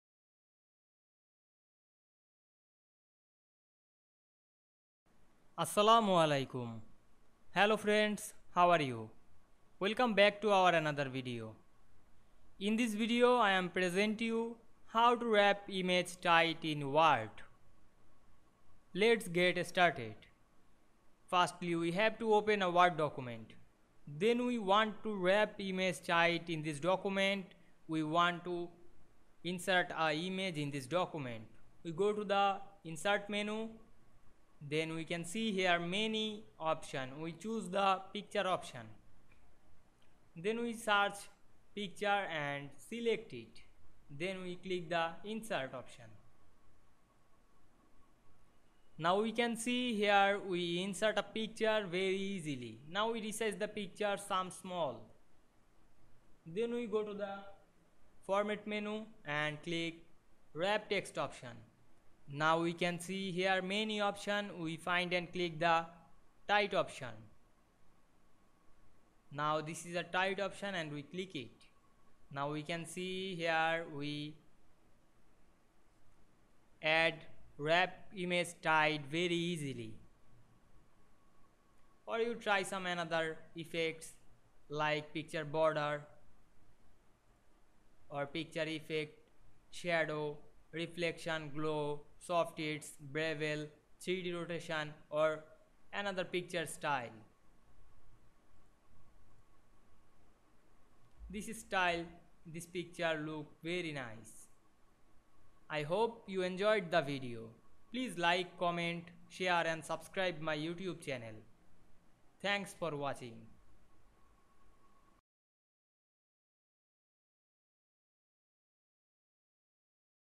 How to wrap image tight in word
Assalamu Walaikum,
In this video I will show you,How to wrap image tight in word. Let's get started.